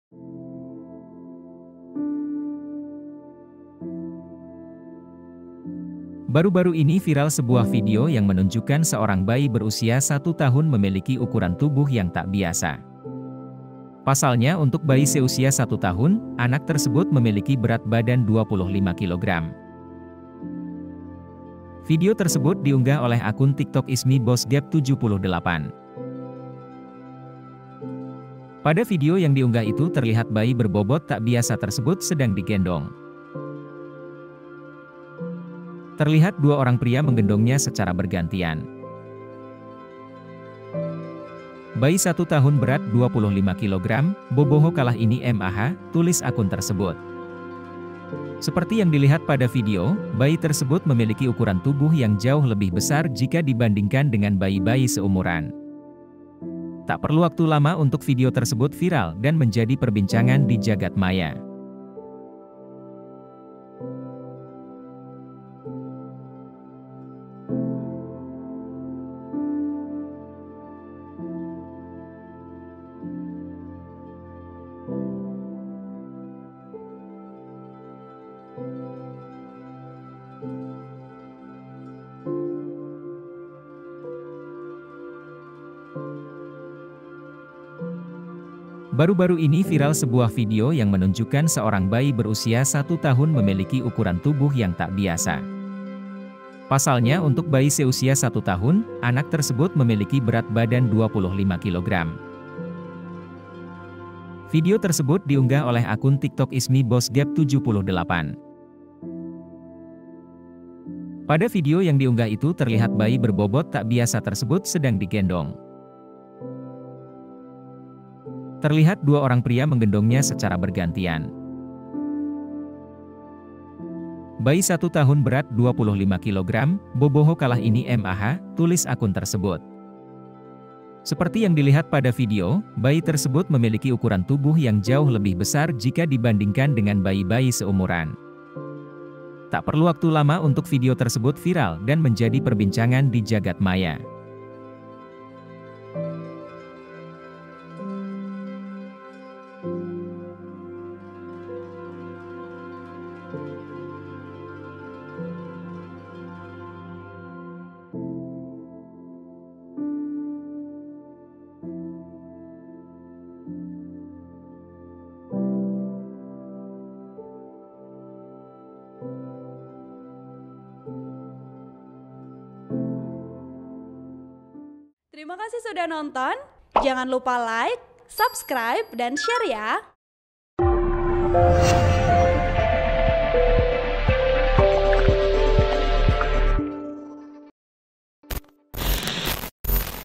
Viral Video Bayi Umur 1 Tahun Miliki Berat Badan 25 Kg, Bikin Warganet Khawatir
POS-KUPANG.COM - Baru-baru ini viral sebuah video yang menunjukkan seorang bayi berusia 1 tahun memiliki ukuran tubuh yang tak biasa. Pasalnya untuk bayi seusia 1 tahun, anak tersebut memiliki berat badan 25 kg. (POS-KUPANG.COM,